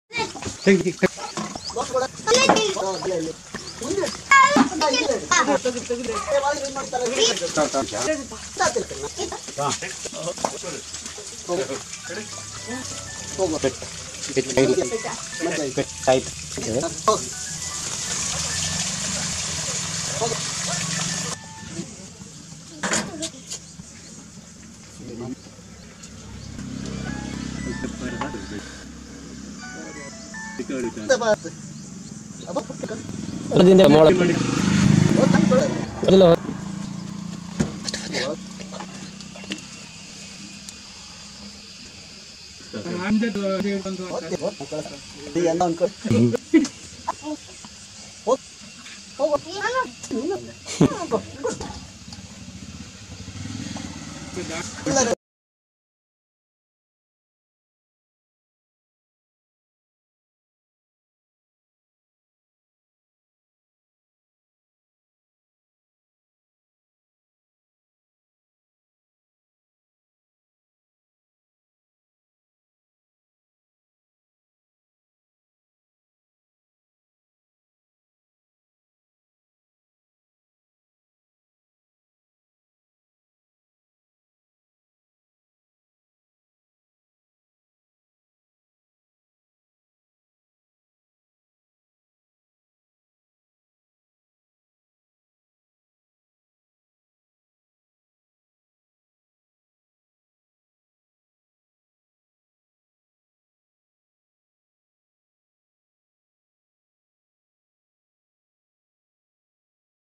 Cobra rescue operation @Siddapura(Udupi Dist)
ಸಿದ್ದಾಪುರದಲ್ಲಿ ಅಪಘಾತವಾಗಿದ್ದ ನಾಗರಹಾವನ್ನು ಸುಧೀಂದ್ರ ಐತಾಳ್‌ ಸಾಲಿಗ್ರಾಮ  ಮತ್ತು ನಿಸರ್ಗ ತಂಡದ ಸದಸ್ಯರು ರಕ್ಷಿಸಿದರು.